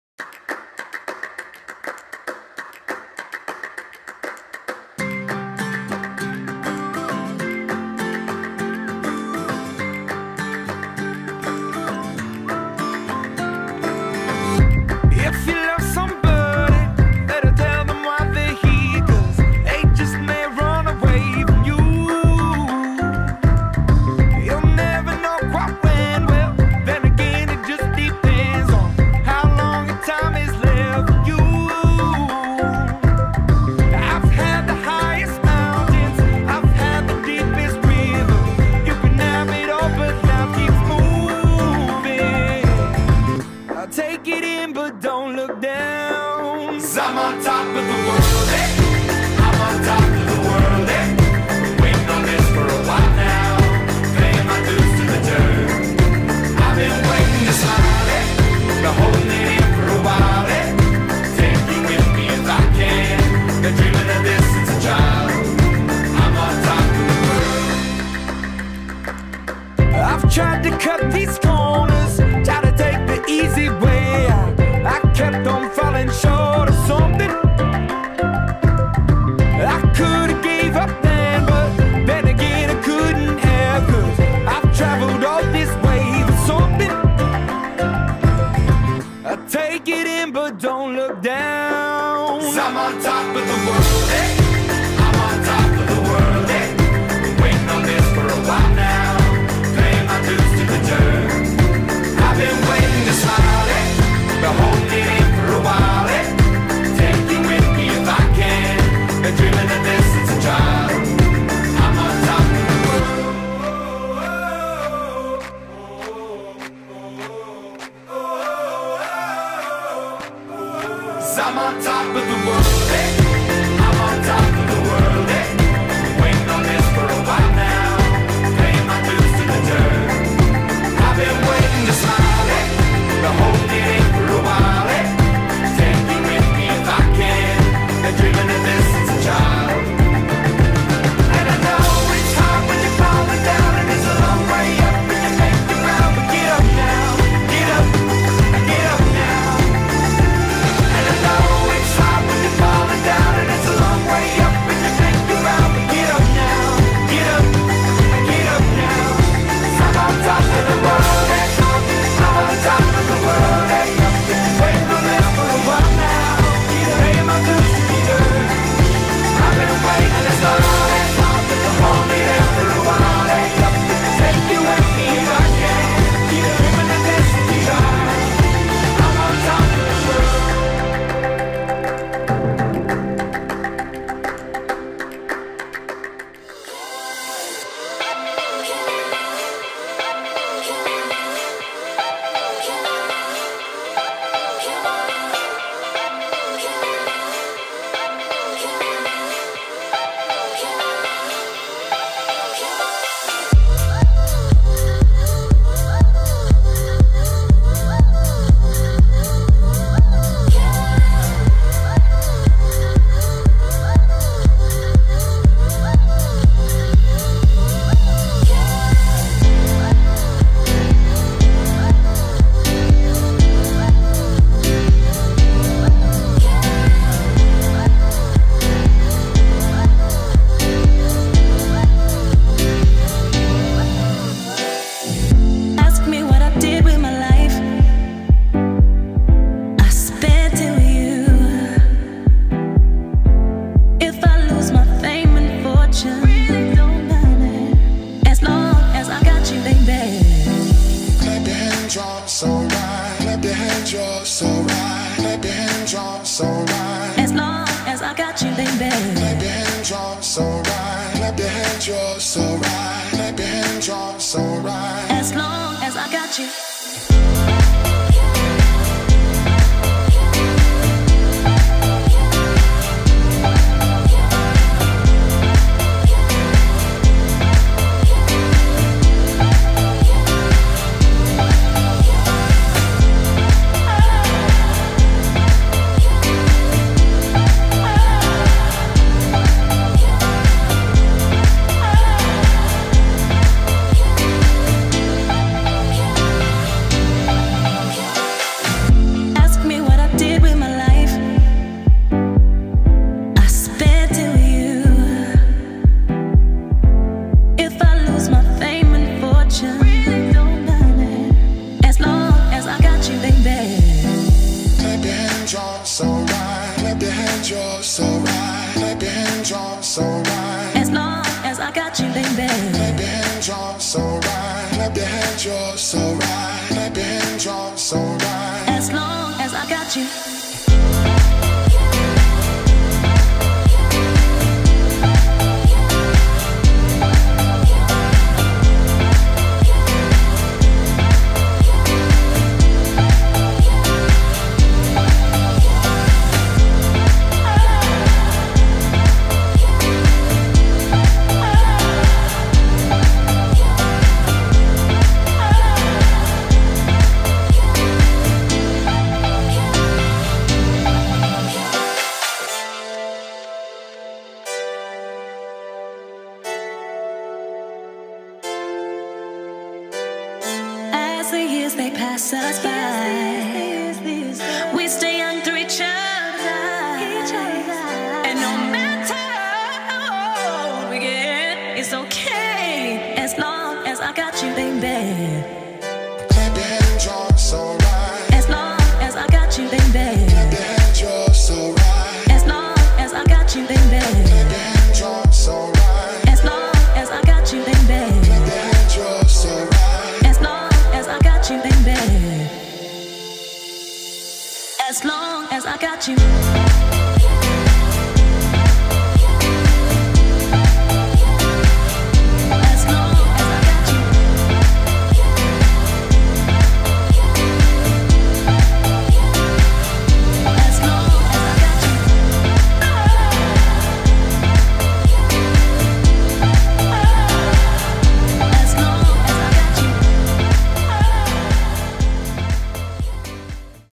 Review of the Year 2013/14
Here's the full review of another incredible year at Whalley Range 11-18 High School. Enjoy.

Music © Imagine Dragons and Duke Dumont